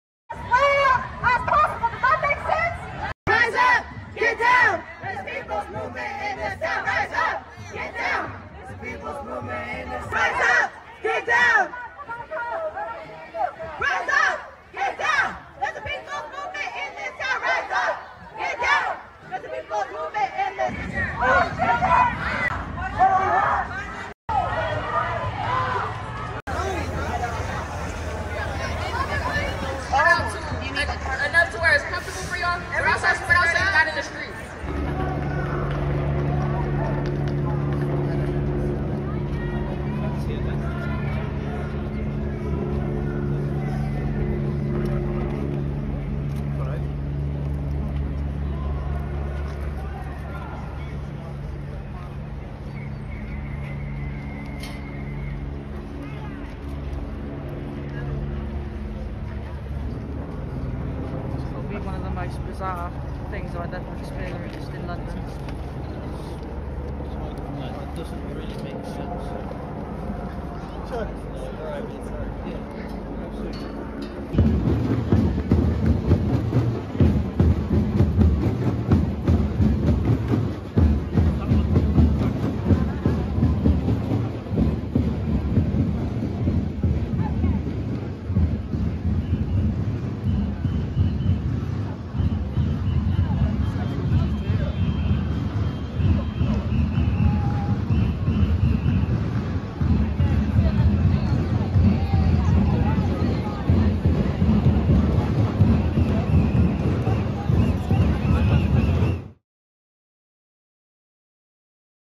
Earth Day 2023: Climate change protesters demand action in US, UK demonstrations
Demonstrators took to the streets of San Francisco to demand action on climate change and other environmental issues on Friday, ahead of Earth Day on April 22.

Chanting "Rise up, get down – the people's movement is in town," demonstrators carried a banner reading: "Youth vs. Apocalypse" as they made their way through the city. The crowd of roughly 100 people then paused to listen to remarks from fellow activists and to watch a dance performance.

Meanwhile, in London, England, protesters staged a “die-in” in on Earth Day, which is Saturday.

The "die-in" was part of a four-day campaign, organized by Extinction Rebellion, dubbed the “Big One,” according to event information.

According to Extinction Rebellion, the event was supported by more than 200 other organizations and companies, including Greenpeace, Patagonia, NHS Staff Voices, Mothers Rise Up, Welsh Rivers Union, and more.

Volunteers in dozens of countries were set to plant trees, clean up trash and urge governments to do more to combat climate change to mark Earth Day, as scientists warn of more extreme weather and record temperatures this year.